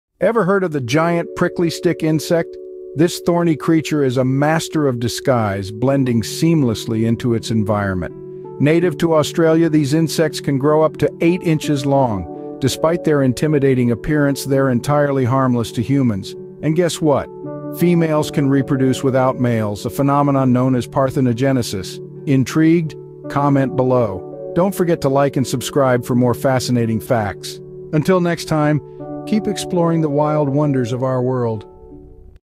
Discover the Giant Prickly Stick Insect: Nature's Master of Camouflage
Description:
🌿 Get ready to meet one of nature's most fascinating creatures - the Giant Prickly Stick Insect! 🦗🌵 Join us on this short journey as we uncover the secrets of this remarkable species. From its uncanny resemblance to a thorny branch to its stealthy camouflage techniques, you'll be amazed by the ingenuity of this master of disguise. Don't miss out on this captivating glimpse into the world of insects! Nature Wildlife StickInsect 🍃